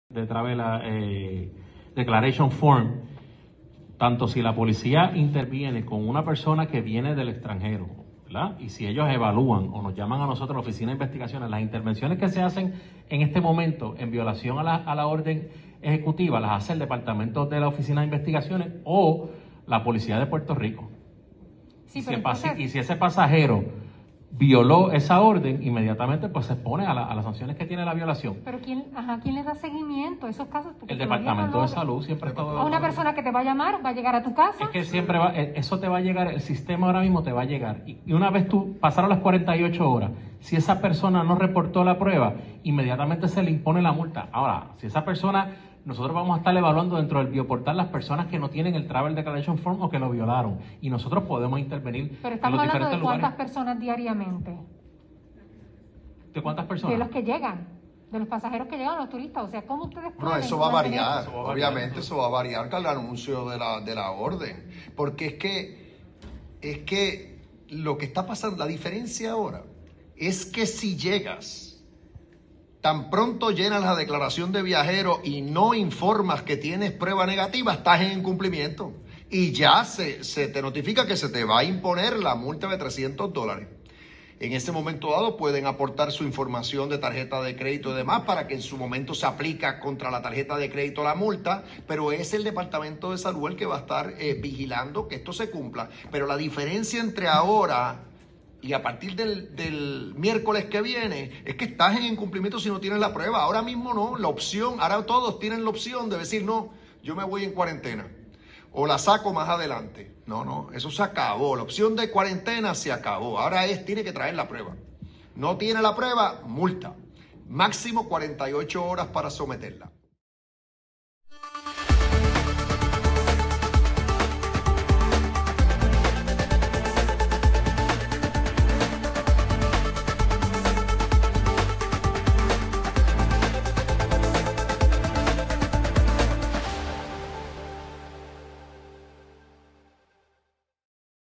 Sansiones a la que se exponen los turistas que lleguen a la Isla y no presenten prueba negativa
CanalMSP Dr. Carlos Mellado, secretario del Departamento de Salud, explica las sansiones a la que se exponen los turistas que lleguen a la Isla y no presenten prueba negativa.